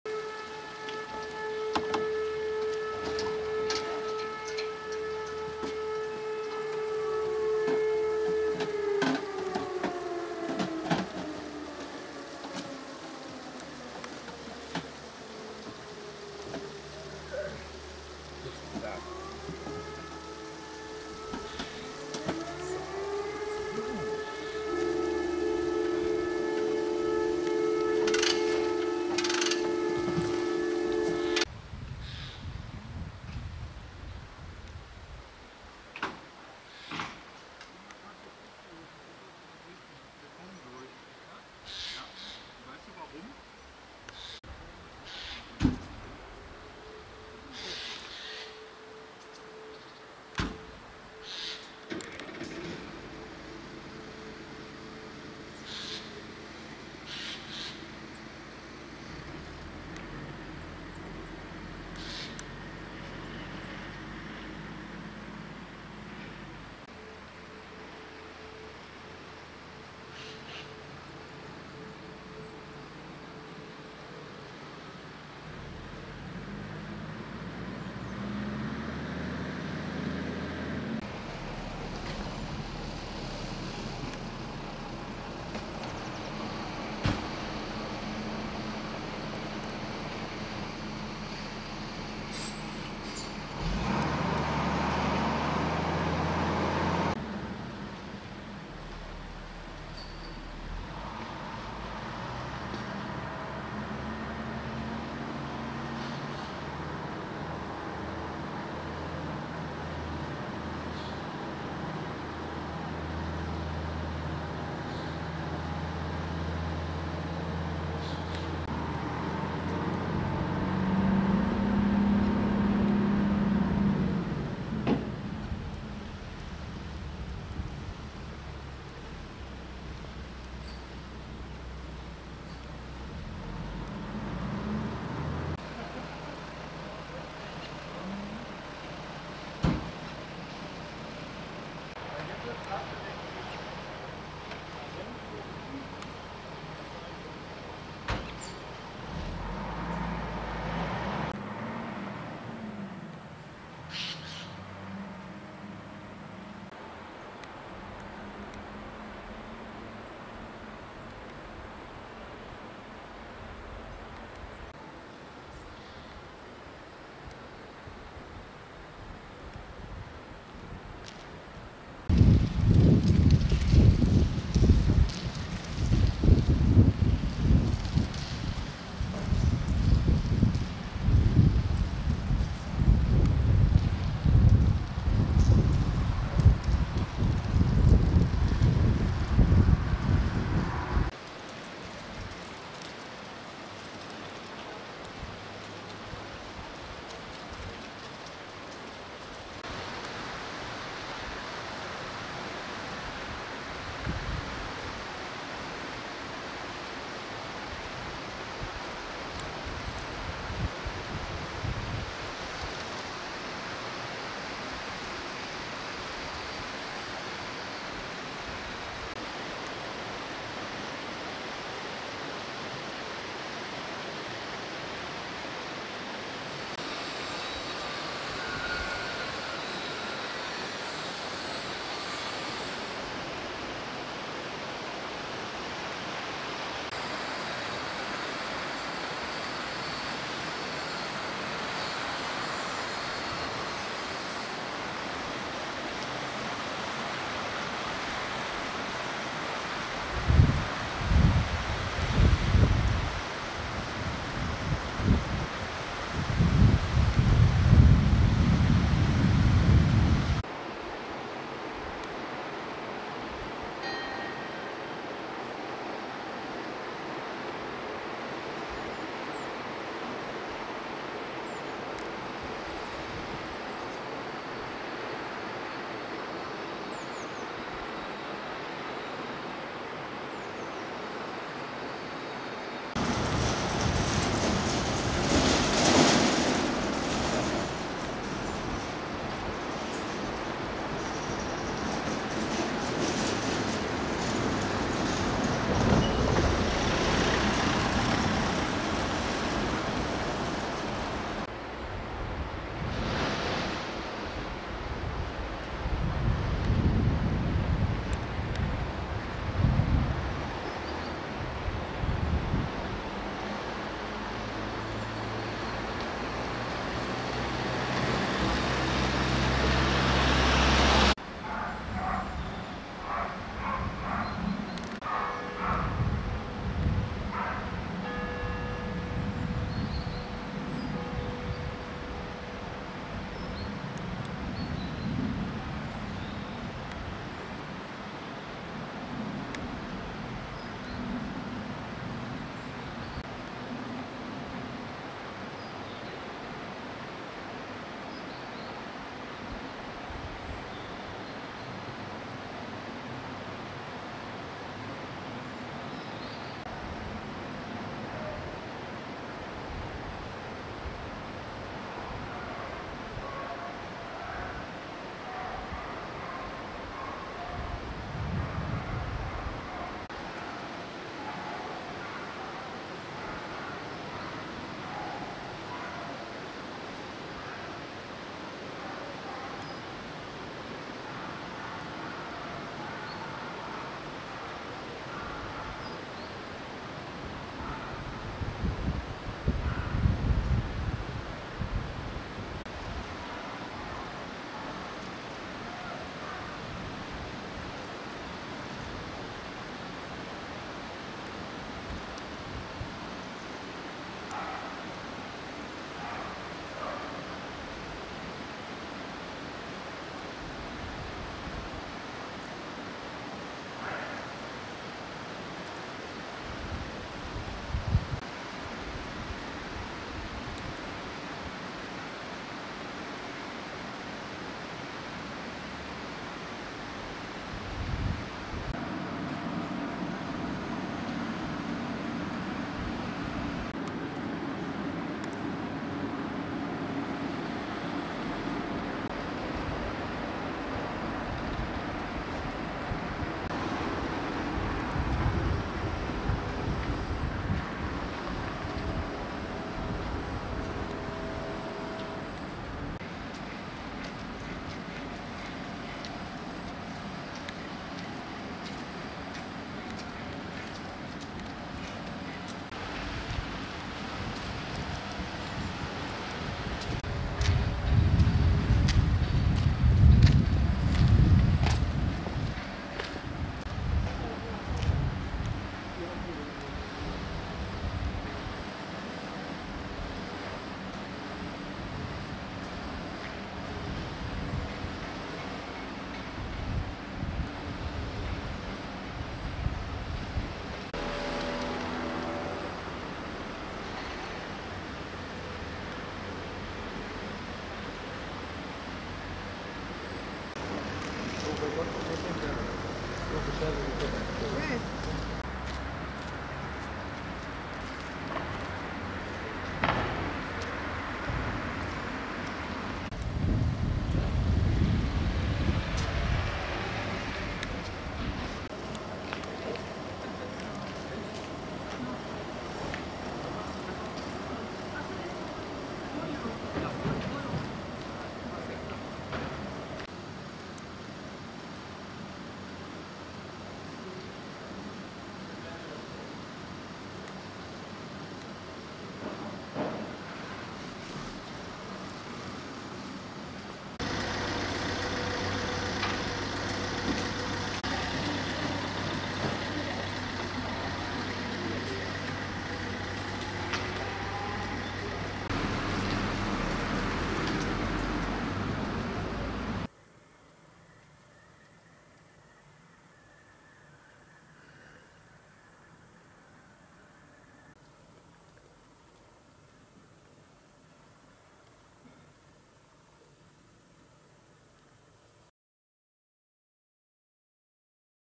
Altenahr Aktuell- 06.11.2021 No Comment
Das große Loch hinter dem Straßentunnel soll bis zum Ende des Jahres wieder gefüllt sein. Bis zum Frühjahr 2022 soll auch die Fahrbahn repariert sein, daßdie Rotweinstraße entlang der Ahr wieder befahrbar ist.